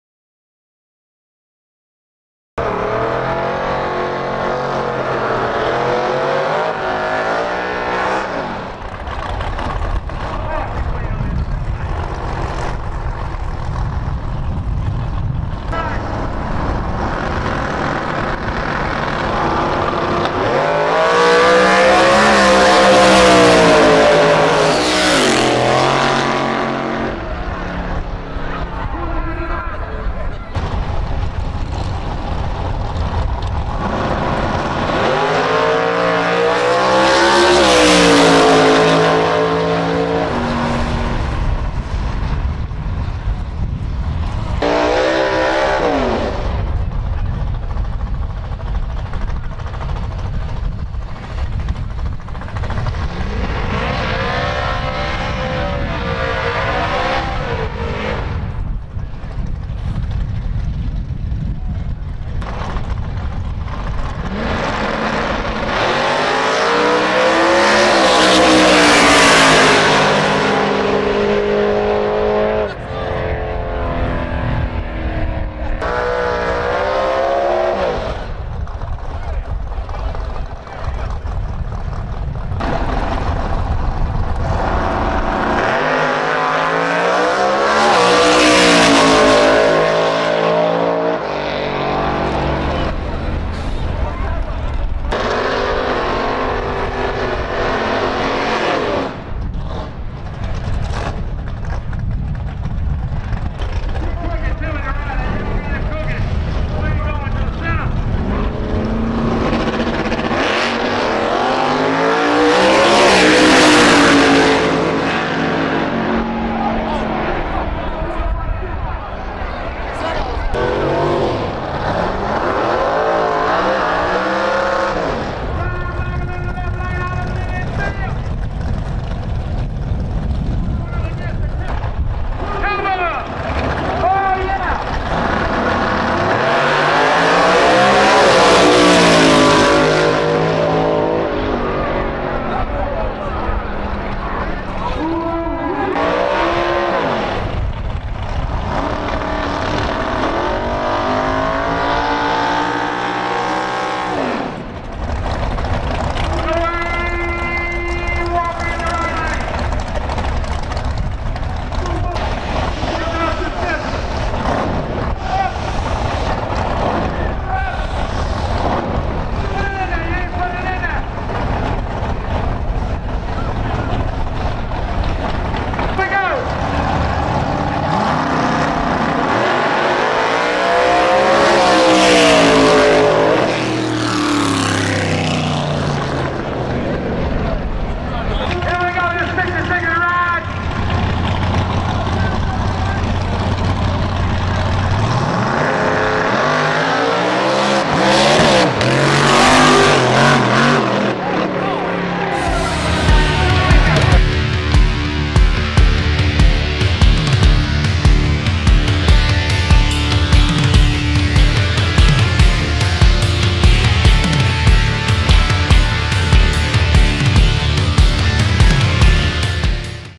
Coupe Performance 2018 (MGMP) Small Blk & Big Blk Heads Up Drag Racing & Grudge Racing (NT) No Time
Thank You In AdvanceCoupe Performance 2018 (MGMP) Small Blk & Big Blk Heads Up Drag Racing & Grudge Racing (NT) No Time
The content on my YouTube channel can't be downloaded, published, edited or used without my permission since it's protected by copyright.